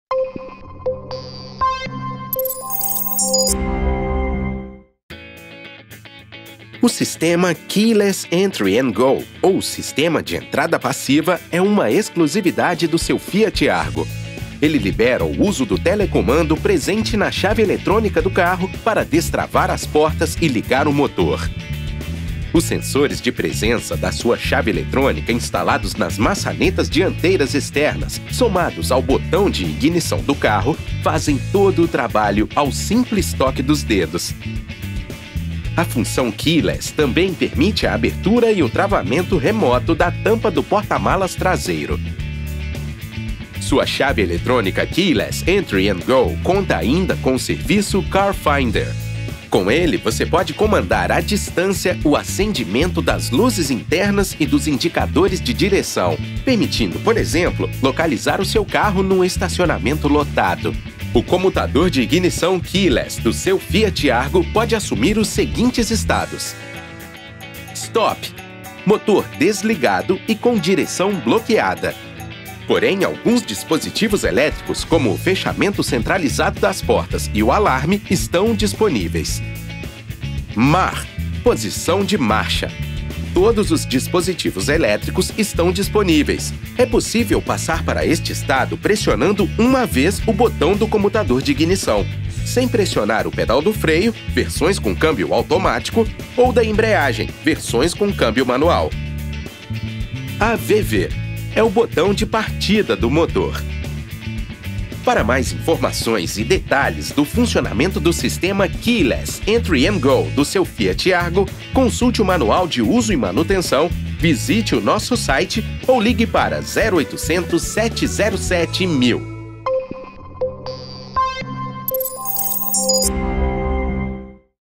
FIAT I Como funciona o sistema Keyless Entry N’ Go do Argo
Exclusivo na categoria, o sistema Keyless Entry N’ Go traz mais segurança e comodidade para você ao entrar e dar partida no seu Argo. Saiba como ele funciona e, para mais informações, acesse